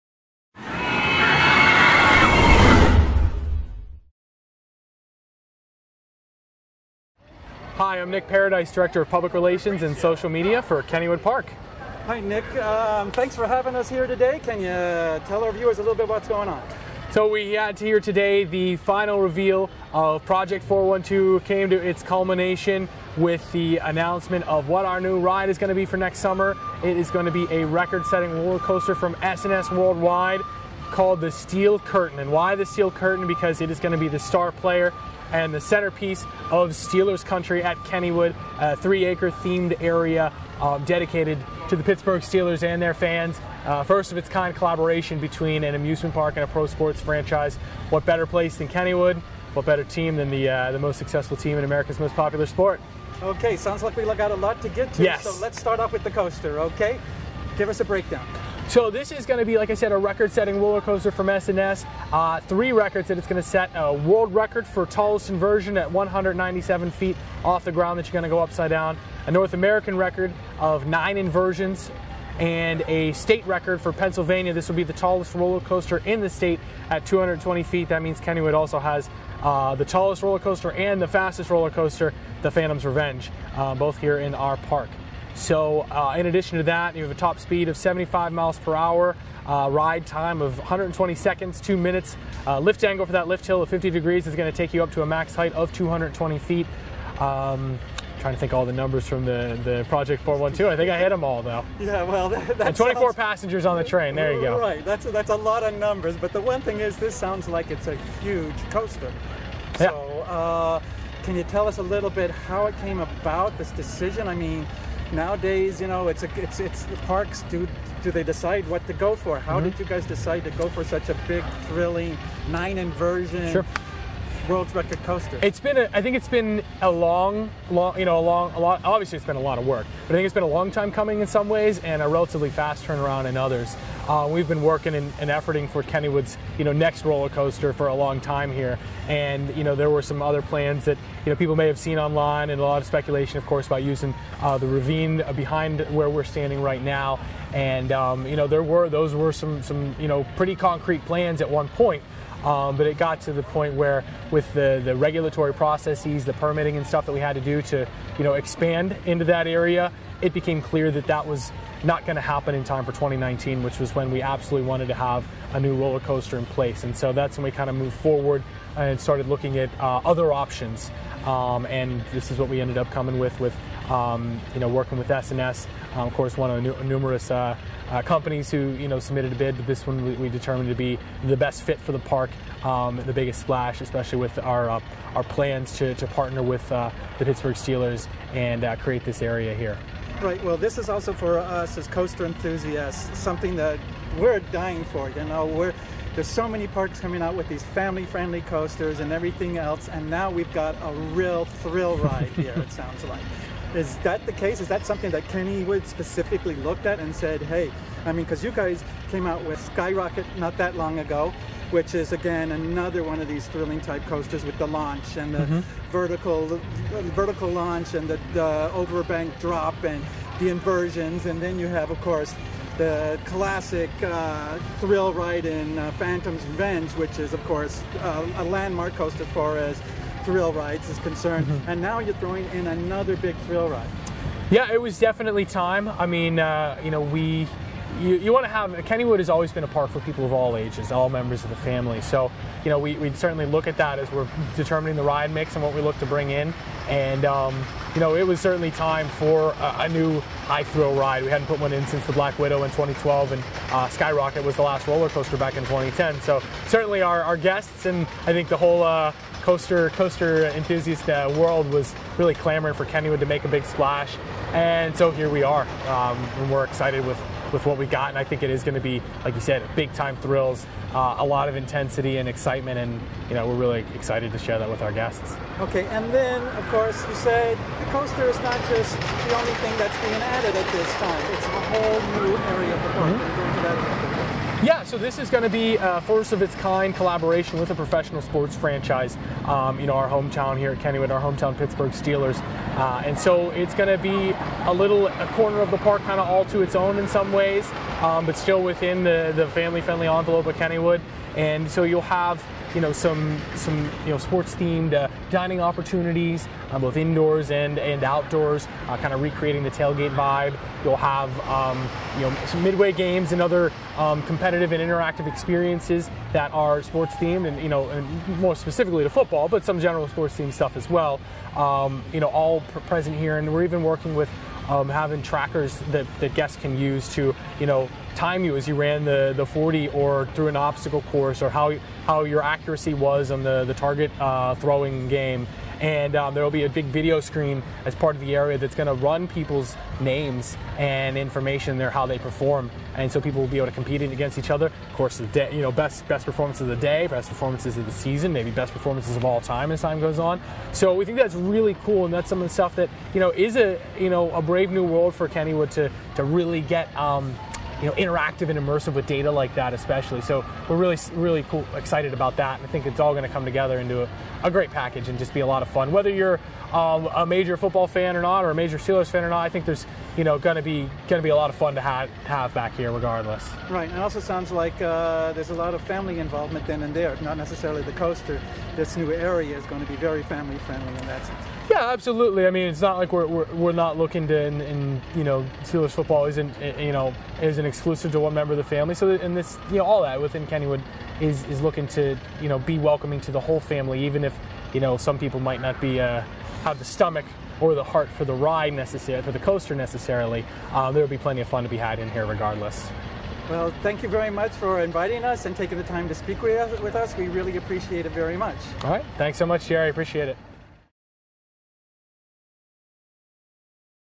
Steel Curtain - Nick Paradise interview Kennywood Project 412
Kennywood Director of Public Relations Nick Paradise, speaks with us about Steel Country and Steel Curtain, the park's new-for-2019 Pittsburgh Steelers themed area and centerpiece S&S custom inverting coaster. Not only will it be the largest coaster the manufacturer ever built, it's also the tallest coaster in Pennsylvania at 220 feet (67m), its 9 inversions are the most of any coaster in North American and it will hold the world record for highest inversion at 197 feet (60m).
Kennywood is located near Pittsburgh on West Mifflin, Pennsylvania.